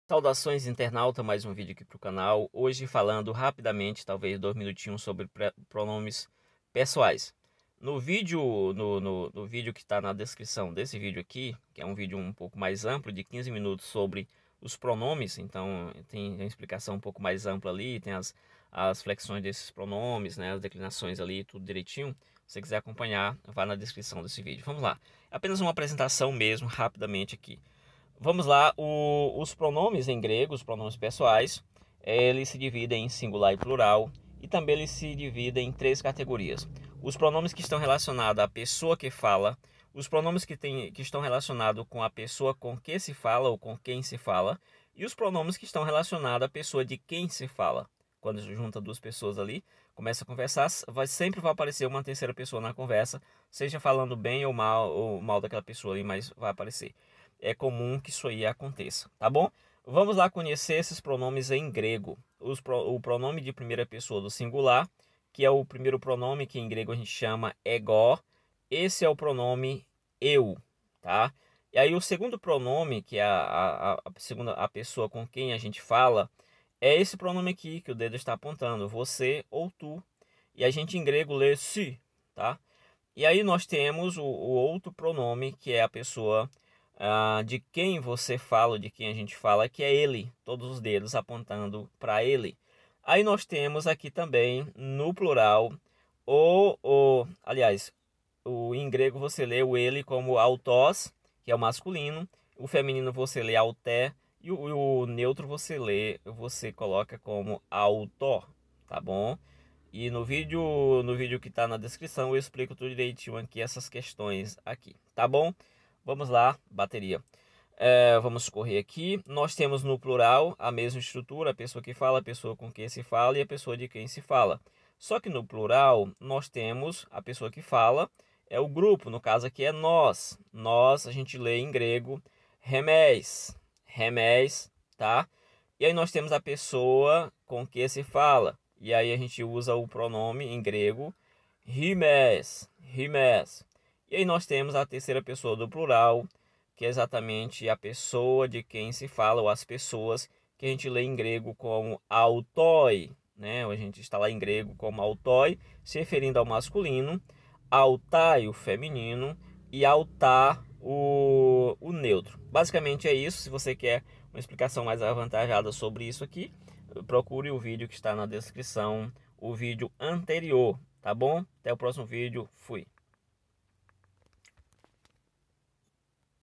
55# - Em 2 MINUTOS. Pronomes Pessoais do Grego Koine.
Este vídeo visa apresentar em 2 MINUTOS os PRONOMES PESSOAIS  do grego koiné/bíblico. Se você quiser consumir um conteúdo mais completo, mostrando as tabelas das declinações de primeira, segunda e terceira pessoa, assista ao vídeo abaixo: